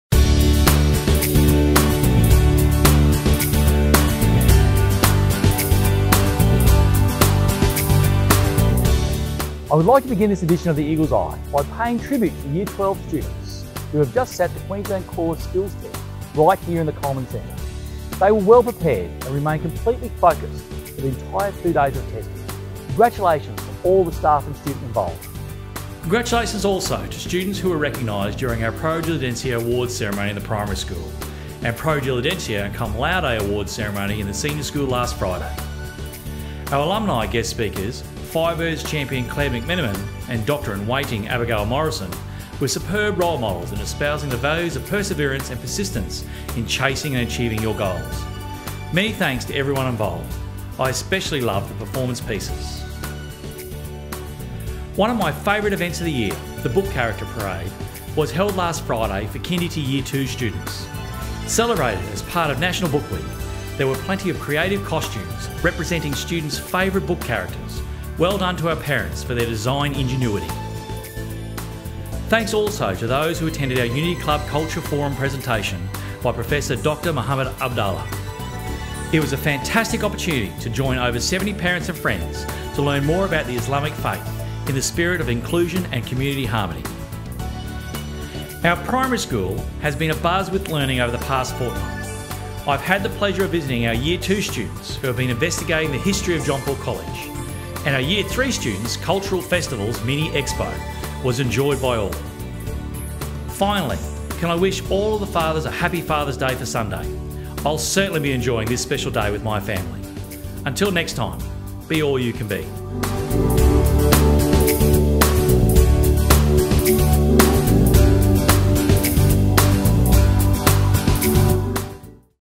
The Eagle's Eye - Edition 013
Welcome to Edition 013 of Headmaster, Peter Foster's fortnightly video-blog.